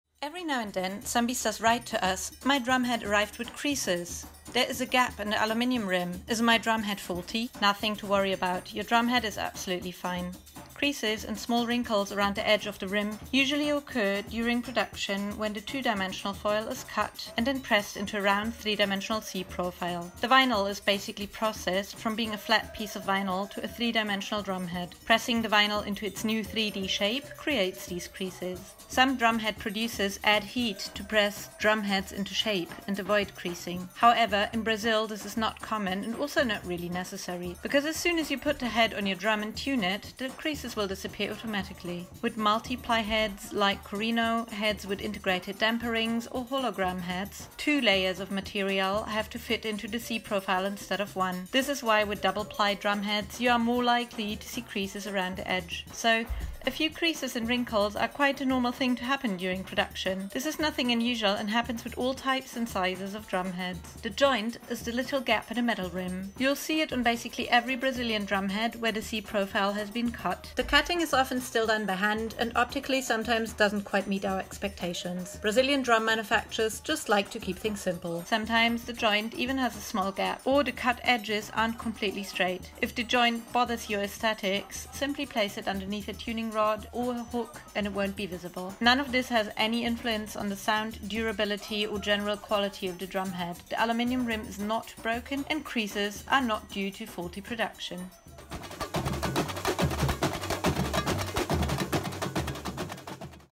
Samba drum head has creases and a gap in the rim - Kalango.com
Every now and then, sambistas contact us with a concerned email "my drum head arrived with creases, there's a gap in the aluminium rim, is the head faulty or even broken?" Nothing to worry about, your drum head is absolutely fine.

Creases or small waves in the drumhead material around the edges of the rim can occur when the two-dimensional synthetic foil is cut and pressed into a rounded, convex, 3-dimensional C-profile during production. During this stage, the vinyl is processed from being a flat piece of  foil to a 3-dimensional object. Pressing the vinyl into its new 3D shape creates these creases. Some manufacturers of drumheads add heat to press the foil into shape and avoid the creasing. In Brazil, however, this is not common and and not really necessary, because as soon as you put the head on your drum and tune it /put tension on it, the creases will disappear automatically. With multi-ply heads like korino, heads with integrated damper rings or hologramme heads, two layers of material have to fit into the C-profile instead of one. This is why with doubly-ply drum heads you are more likely to see creases around the edge.

So, creases a a normal thing to happen during production. This is nothing unusual and happens with all types and sizes of drum heads. The joint is the little gap you see on basically every Brazilian drum head, where the C-profile has been cut. The cutting is often still done by hand and optically sometimes doesn't quite meet our expectations. Brazil just likes to keep these things simple :) Sometimes the joint shows a small gap, or the cut edges aren't completely straight. If the joint bother's you aesthetically, simply place it underneath a tuning rod or a hook and it won't be visible. None of this has any influence on the sound, durability or general quality of the drum head. The aluminium rim is not broken and creases are not due to faulty production.